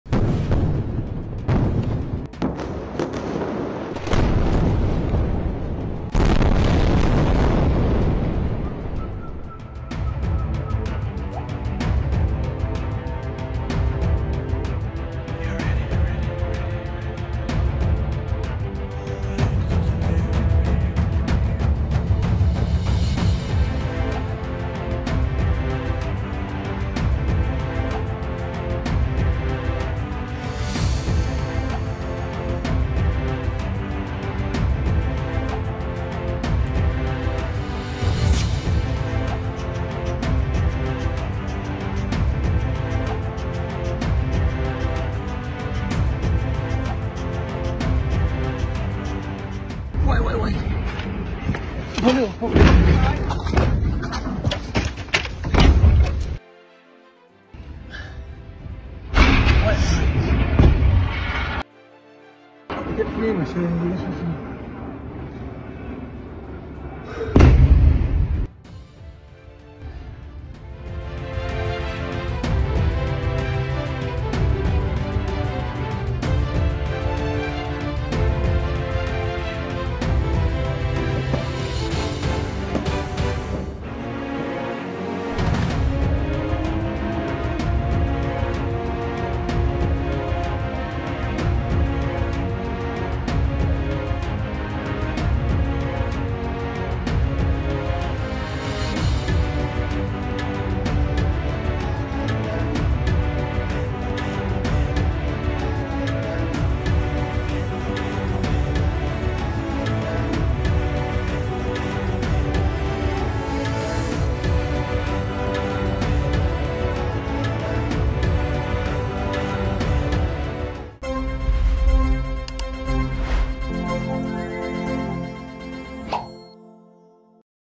Fauda Star Lior Raz Takes Cover From A Rocket As He Joins Forces In Israel War Against Hamas
Israeli actor Lior Raz is best known for his character Doron Kabilio in hit show Fauda. But has now taken to the front lines of Israel's war against Hamas. Lior Raz has joined a group of volunteers 'Brothers in Arms'.